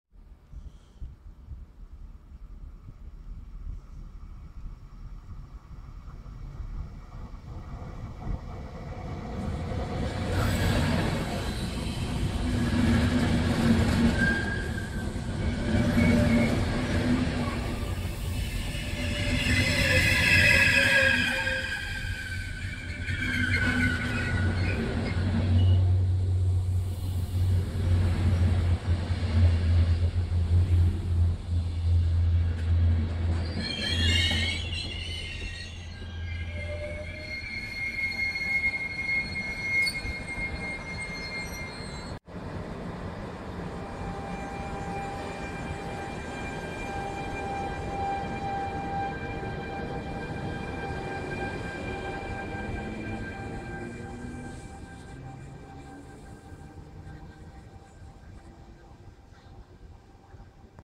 E464 413+7 piano ribassato Trenord - Milano Greco - 10/12/2021
R25846 Bergamo-Milano Porta Garibaldi effettuato con 7 carrozze a piano ribassato con E464 413 in coda.